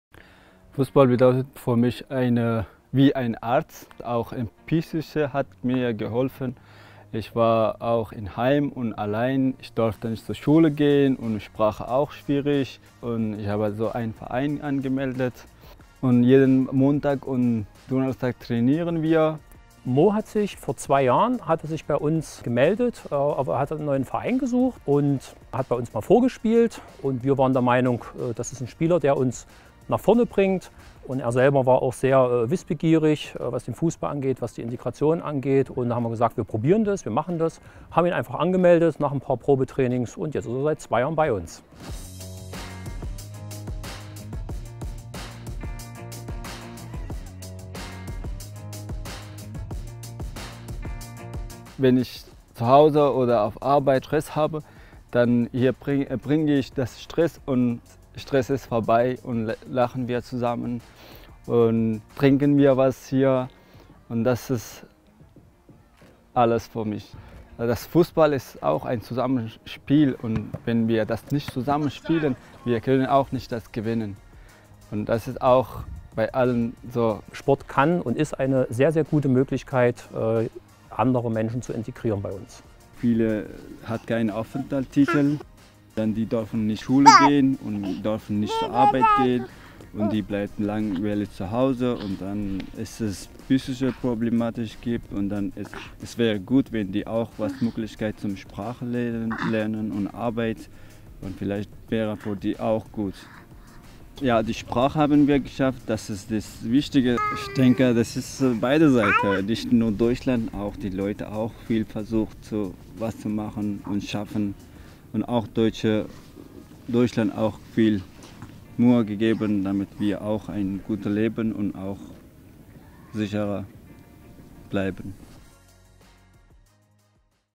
Integration durch Sport: Mohammeds Geschichte
„Fußball ist mein Arzt,“ sagt der 28-Jährige Mohammed aus Afghanistan. Er kam 2015 nach Deutschland und fand über seinen Verein BSV Halle Ammendorf viele Freunde. Inzwischen macht er eine Ausbildung zum Altenpfleger und ist Vater einer kleinen Tochter.